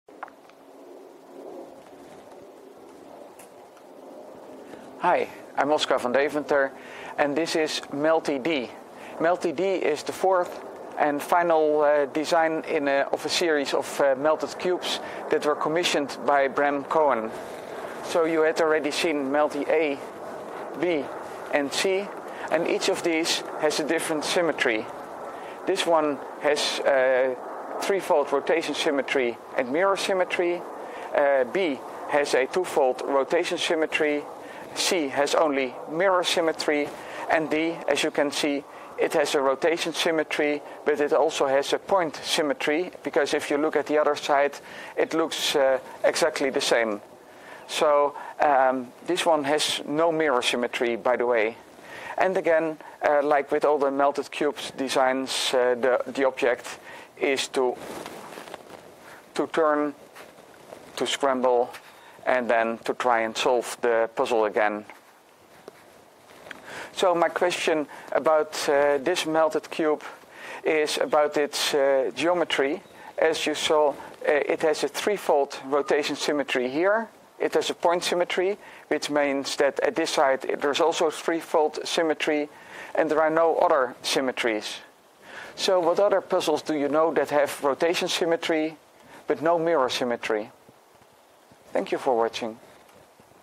Melty D
Melty D is the fourth of a series of four Melted Cube designs proposed by Bram Cohen. Whereas the six axes in a regular Rubik's Cube are at 90 degrees, they are at 85 degrees in de Melted Cubes. The six axes of Melty D can be described as follows. Place three axis with 85 degrees between each pair. Place a second set of three axes with also 85 degrees between them such that the three each have an 85 degrees angle with one of the first set.

The result is an interesting puzzle, that can be fully scrambled according some definitions, but not according to other.

Melty D has threefold rotation symmetry, point symmetry around its origin and chirality. This makes Melty D the most symmetric among the set of four.